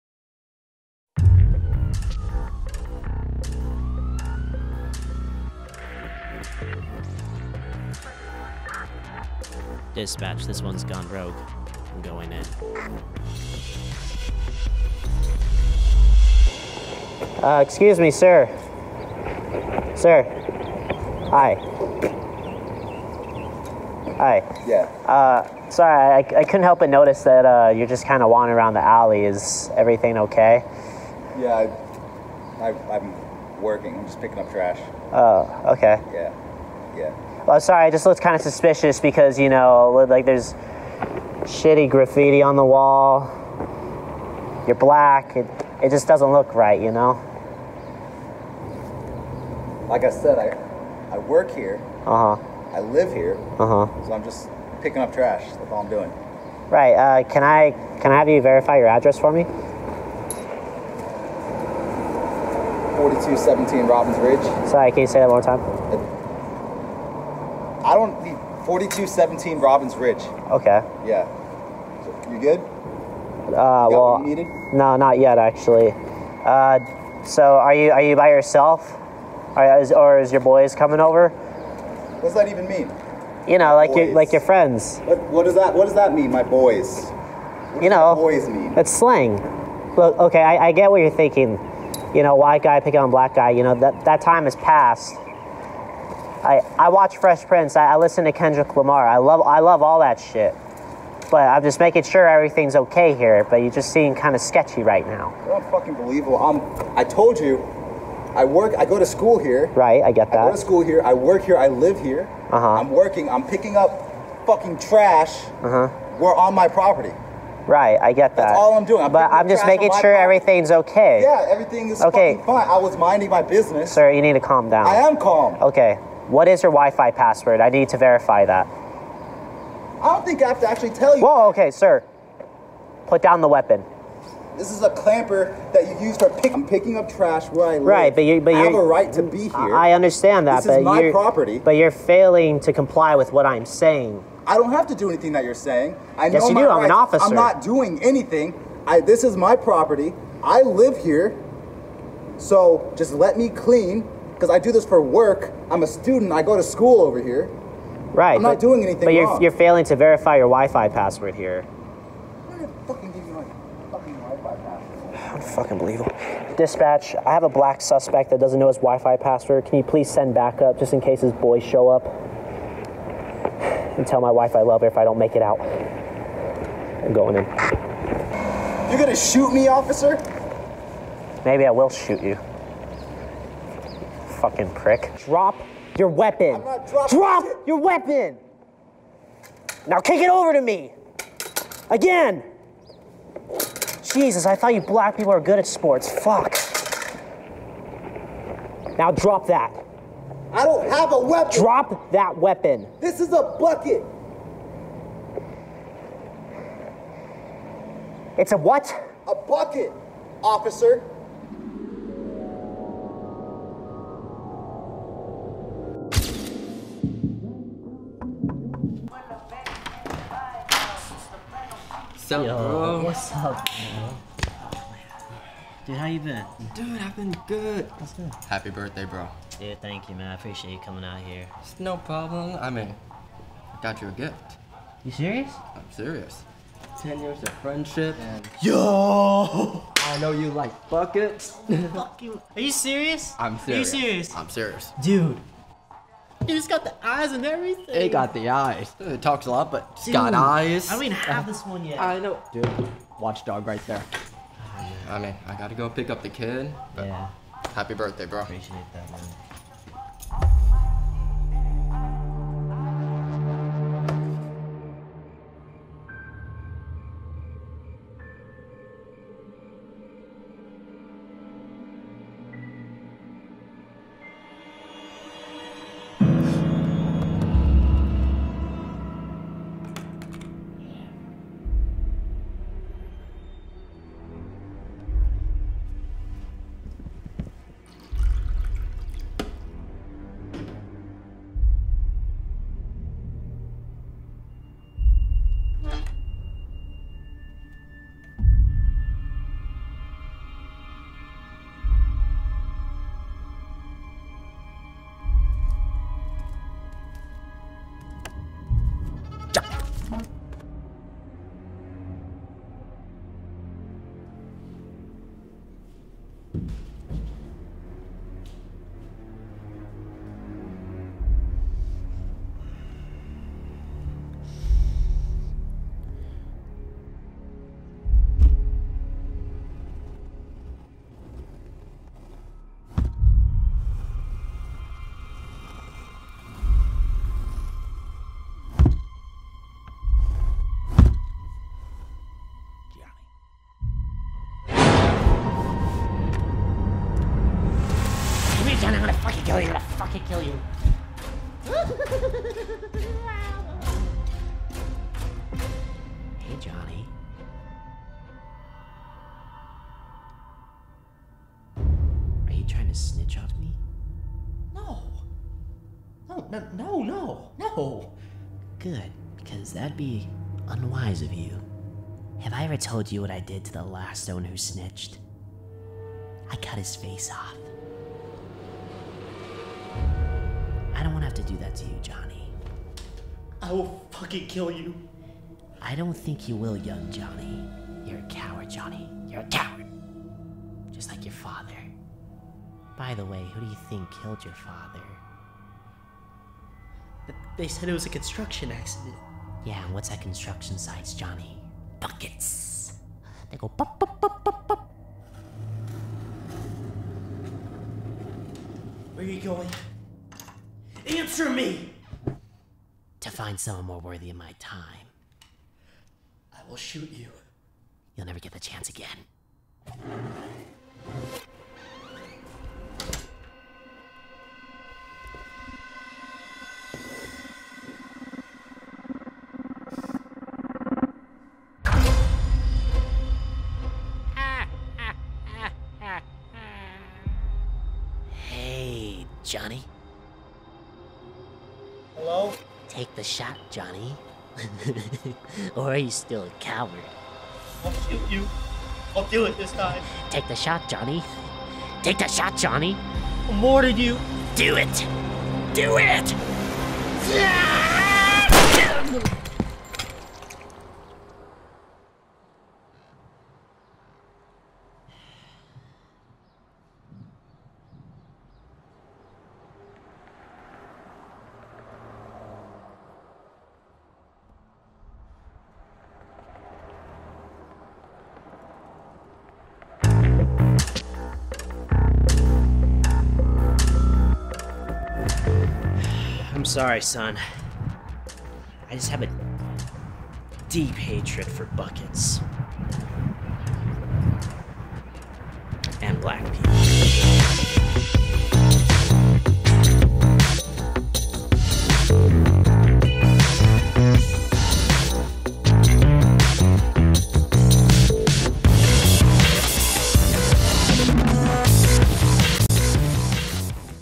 Police Officer Harasses Student Cleaning Around His Dorm
A month ago I was cleaning garbage around my house when a police officer approached me saying I was trespassing, at first I was very uncomfortable but then I found out I wasn't the only one being harassed...

Starring: Dillon Wrich as Officer John and Bucky the bucket

Follow Happyfox Productions on Instagram!

If you listen to Kendrick Lamar does that make you not racist?